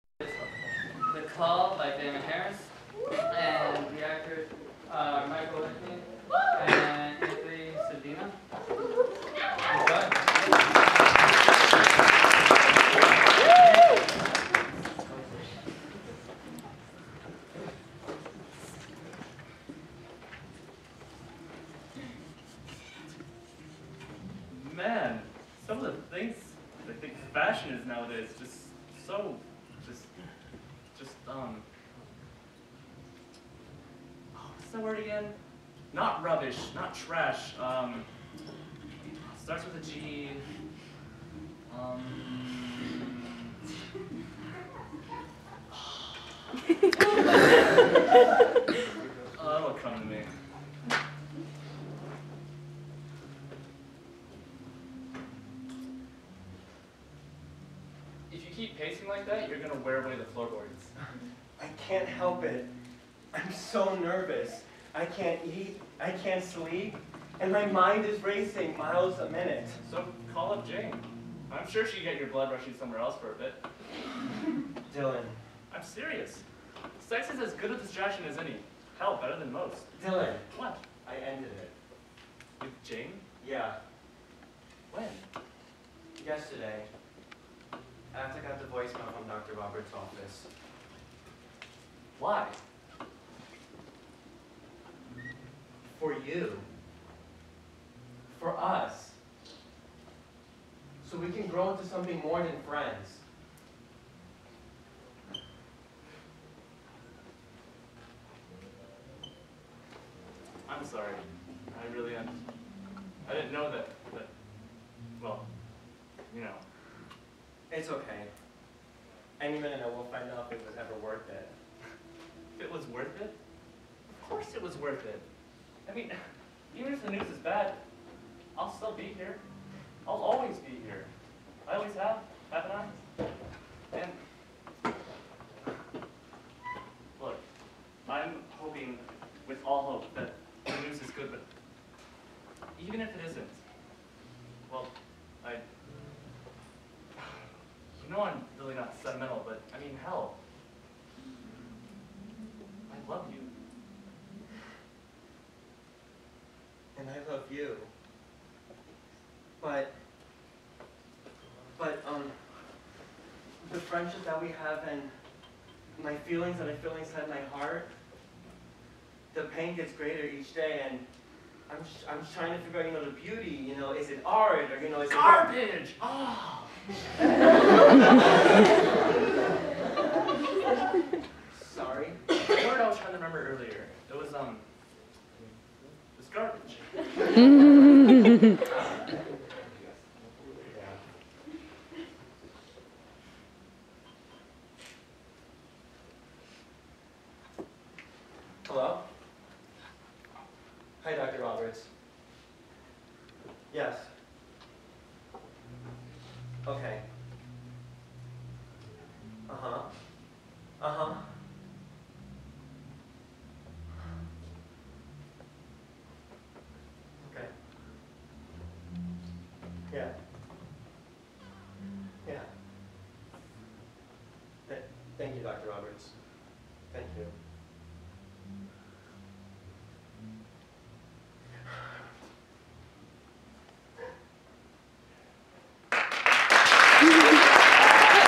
The Call.MTS
*I apologize for the shaky camera and my camera's horrible auto-focusing*

The Call

Written by Dana Harris
Directed by Jon Ugalde
Performed by Anthony Cedeno and Michael Whitney

at Alfredian Dramatists 24 Hour Theatre Marathon 9/15/12